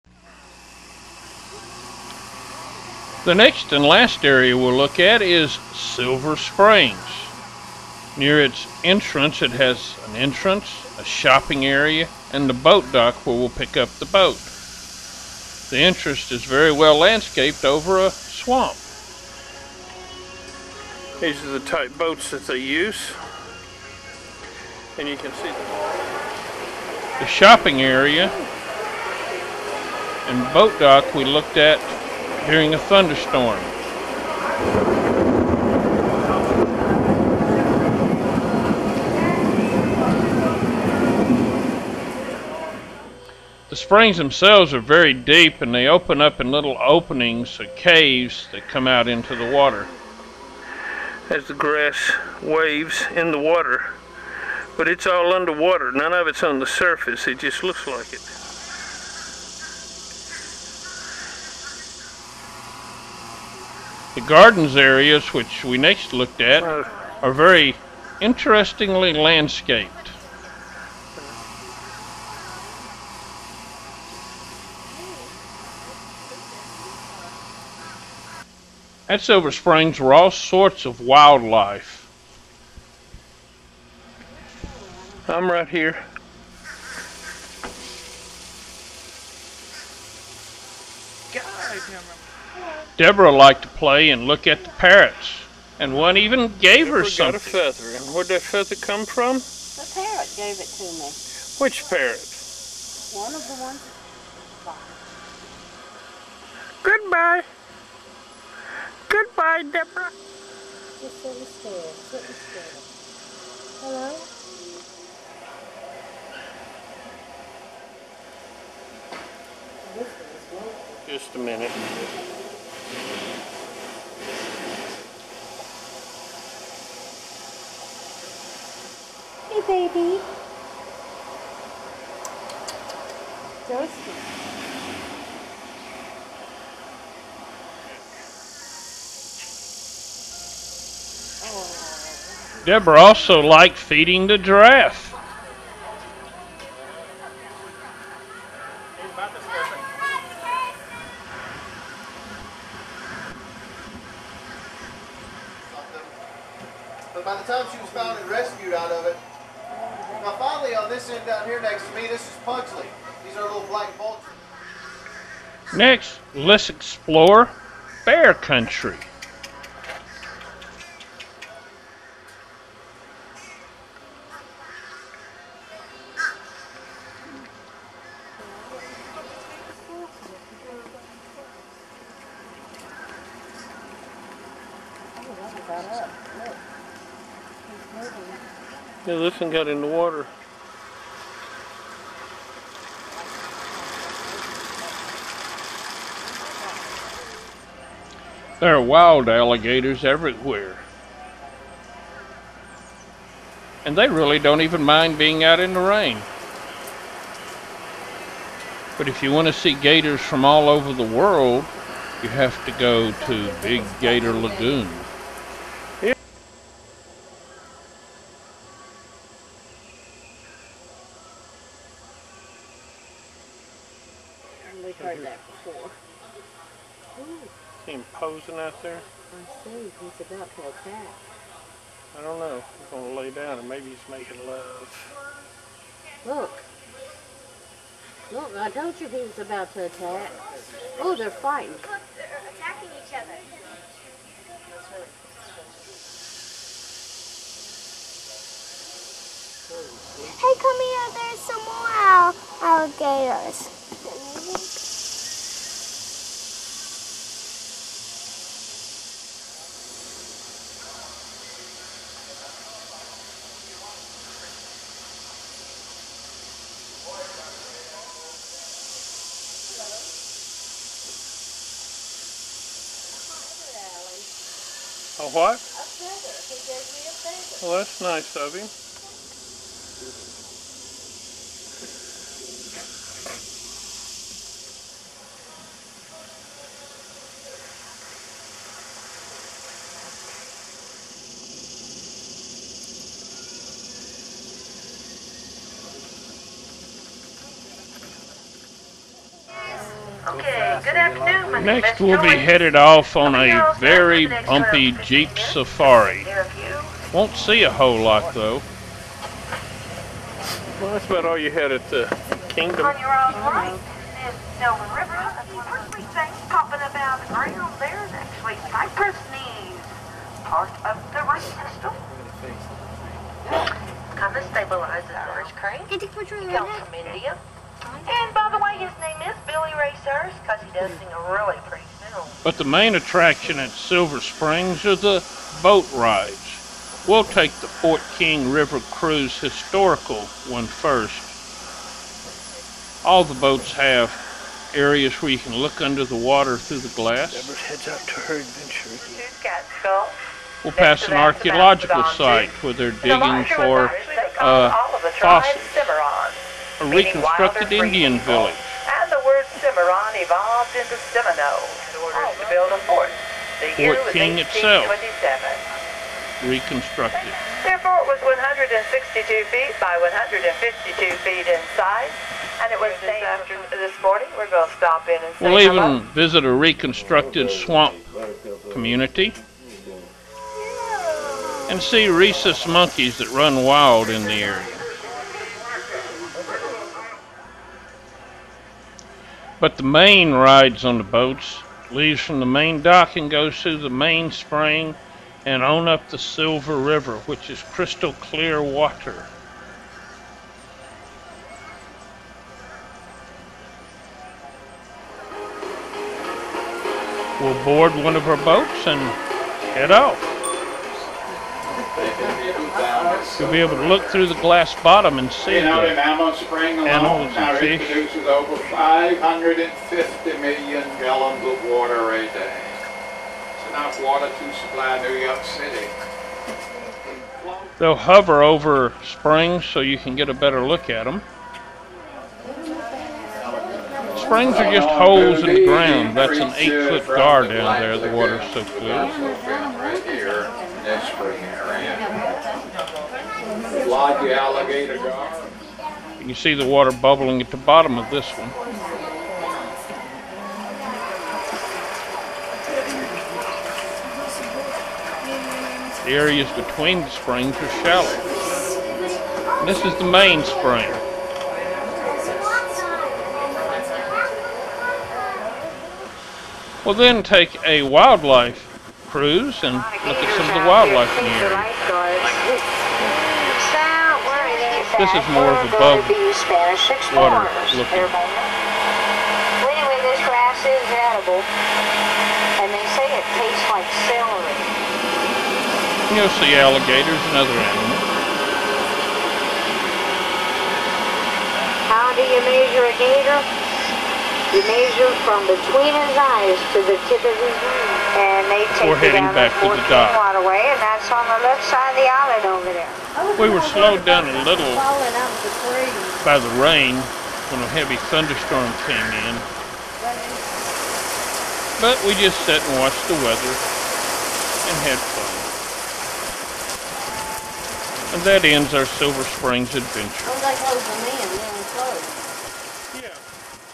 2005 - Orlando - Silver Springs
This is video of a trip to Silver Springs in 2005 on the way back from Orlando